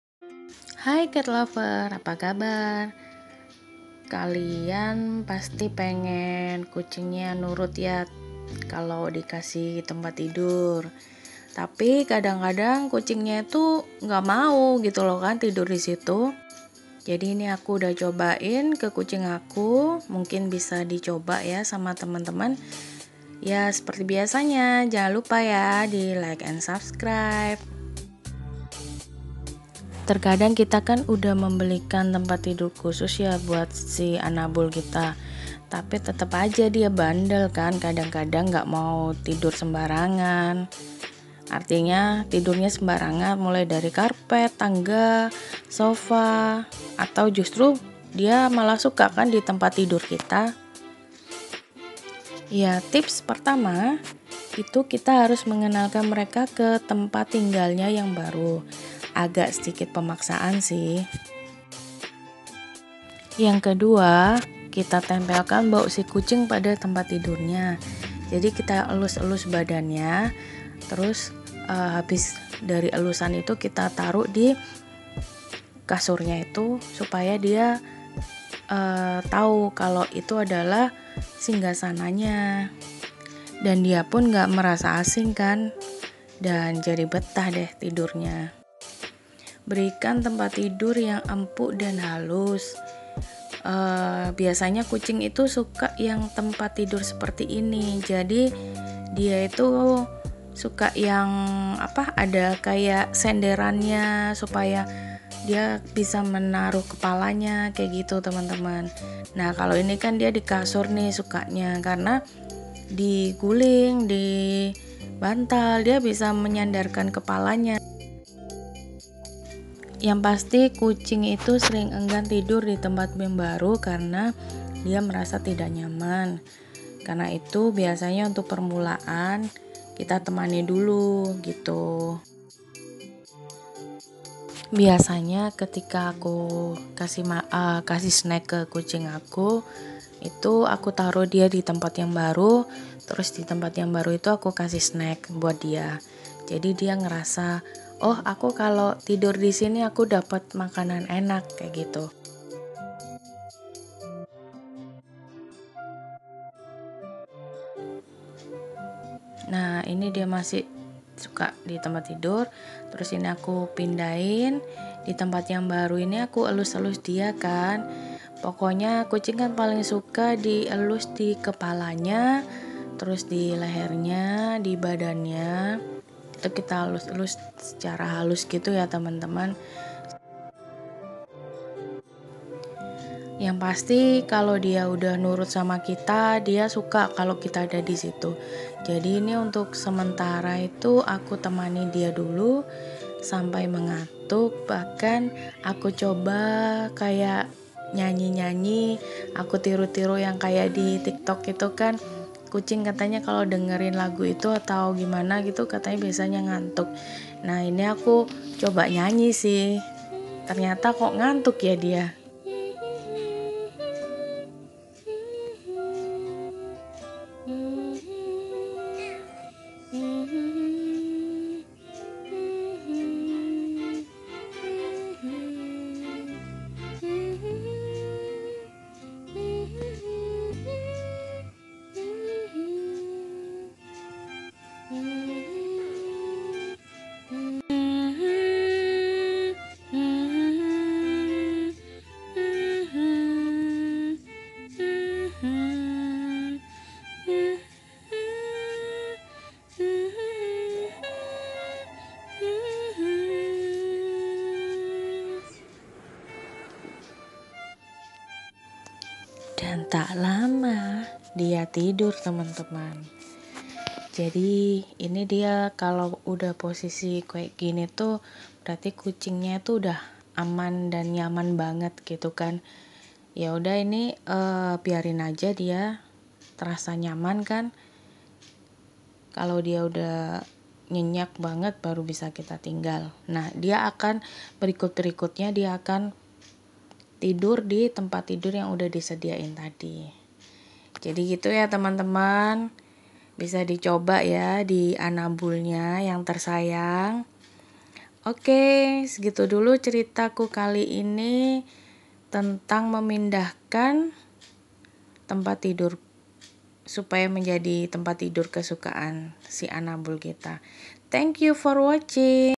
Cara kucing supaya tidur ditempat tidur nya
Tips untuk kucing supaya mau tidur ditempat yg disediakan ..